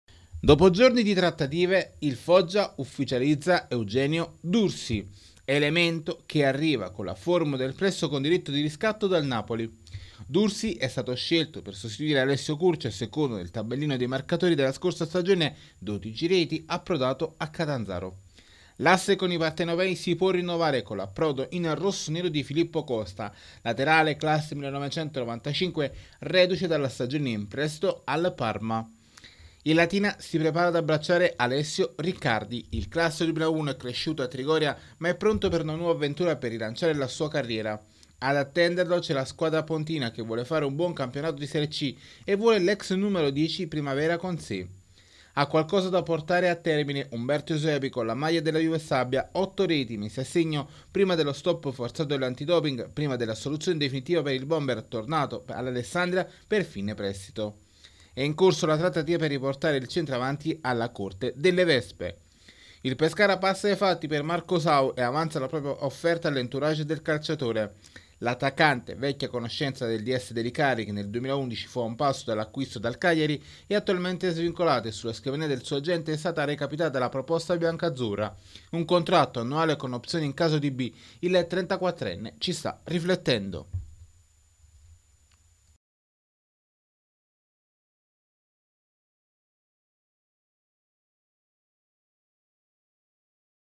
Foggia dopo D'Ursi c'è Costa, Riccardi al Latina
Juve Stabia a lavoro per il ritorno di Eusepi dall'Alessandria. Il Pescara tenta il colpo da novanta con 'l'offerta avanzata a Marco Sau